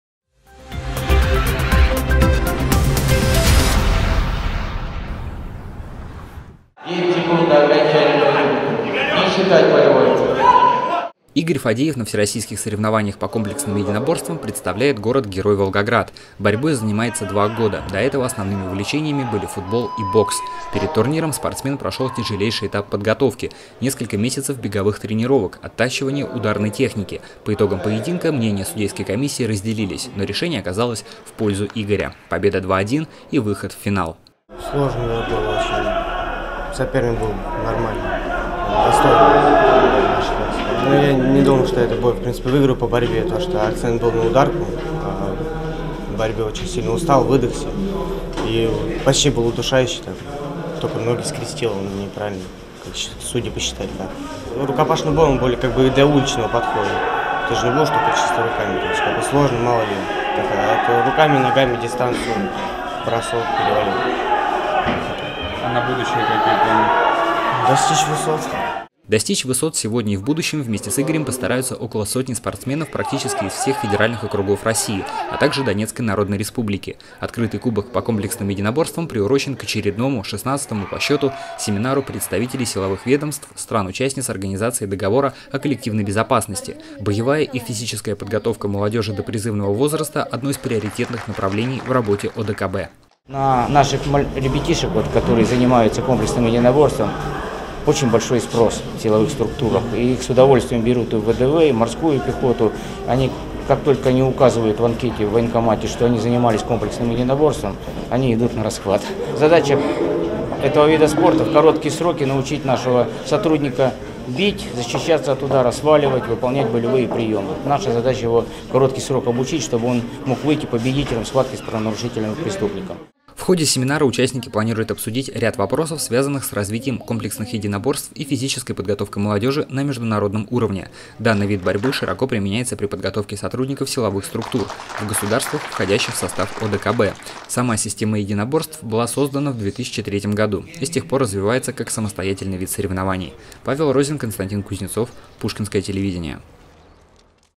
Открытый кубок ДОСААФ
В среду, 3 апреля, в Российском Государственном Университете Туризма и Сервиса состоялась церемония открытия 16-го международного семинара-совещания представителей силовых ведомств государств-участниц Организации Договора о Коллективной Безопасности. В регламенте заседания предусмотрено проведение Открытого кубка по комплексным единоборствам. На соревнования заявились 12 команд и около 100 спортсменов из разных федеральных округов России и соседних республик